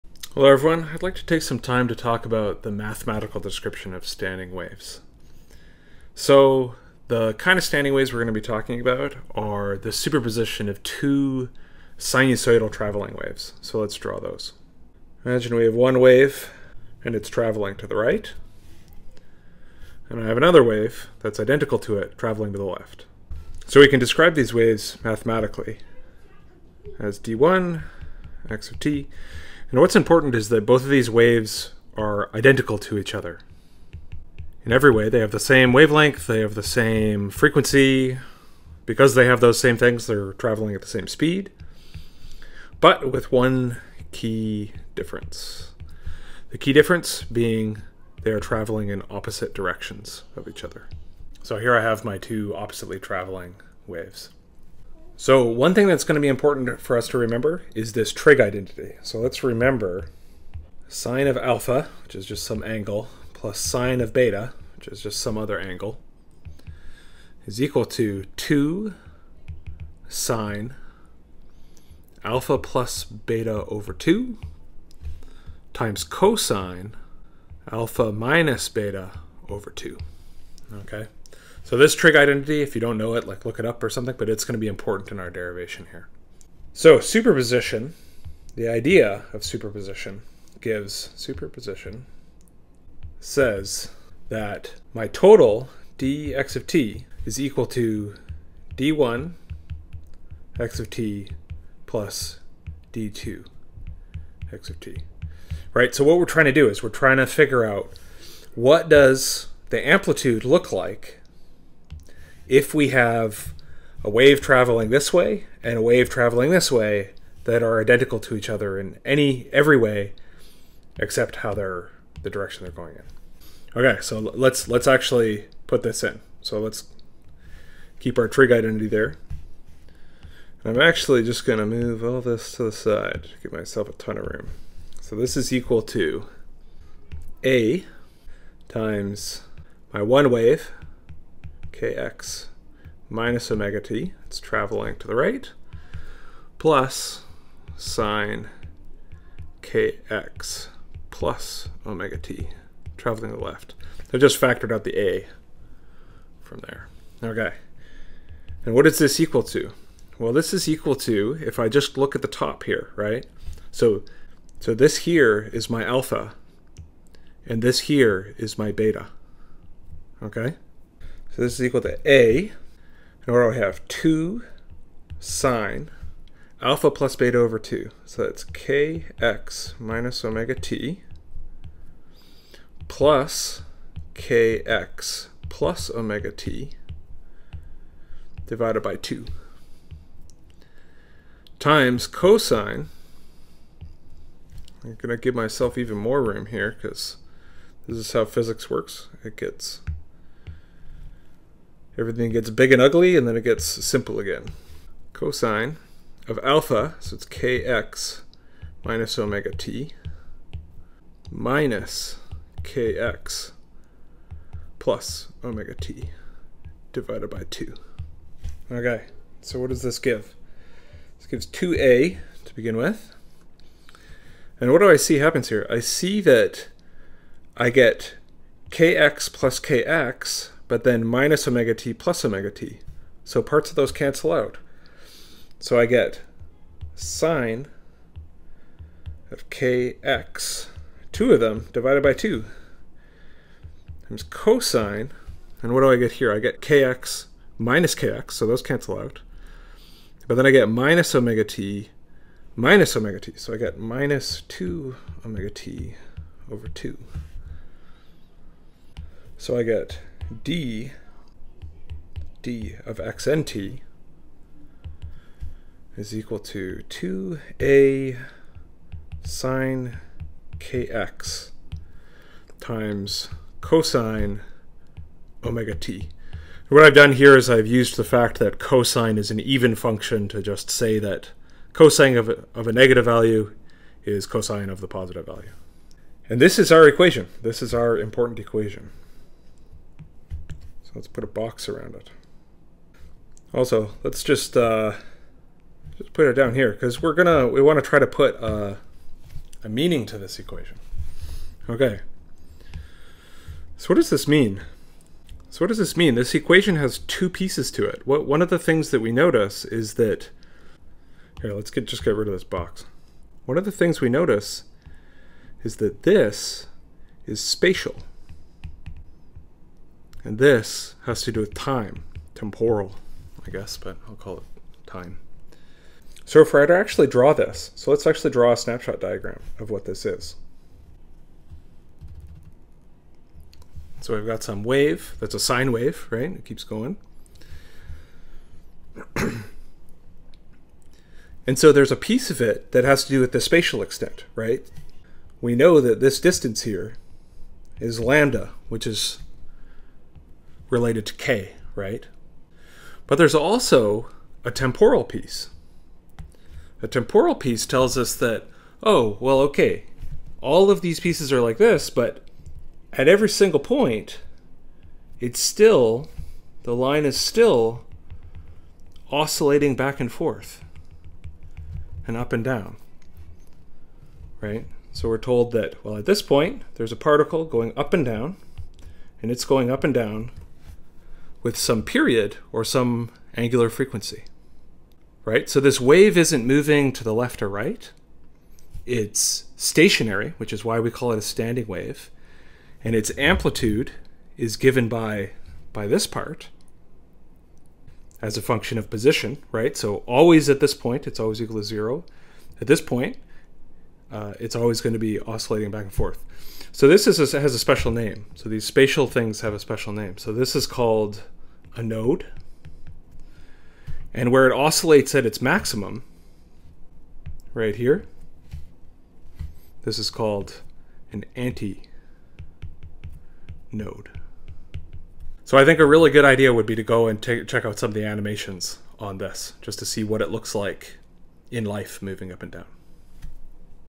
Mathematical Description of a Standing Wave
We derive the mathematical equation for a standing wave by considering the superposition of two identical travelling waves that are moving in opposite directions. We then try to give the equation some physical meaning.